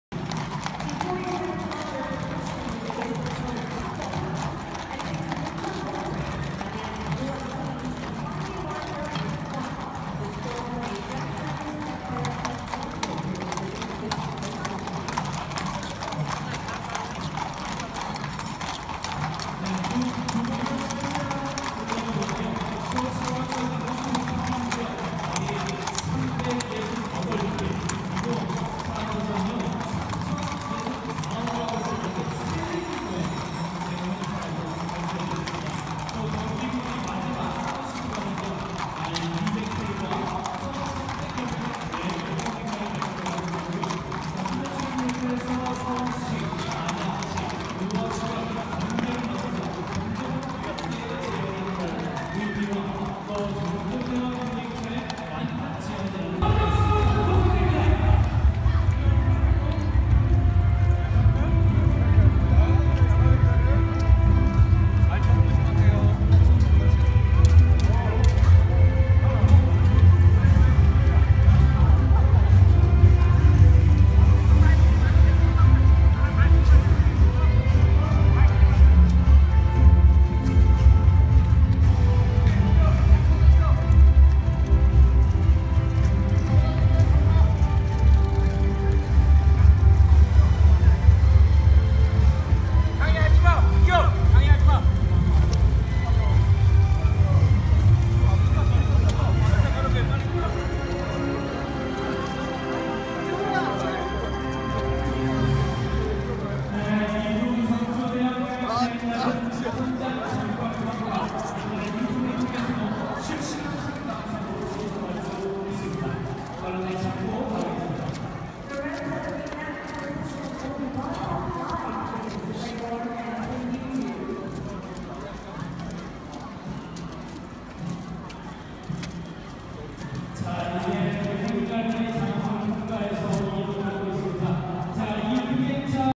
朝鲜正祖大王巡幸陵墓场景再现
朝鲜正祖大王巡幸陵墓场景再现！巡幸队伍进入水原华城长安门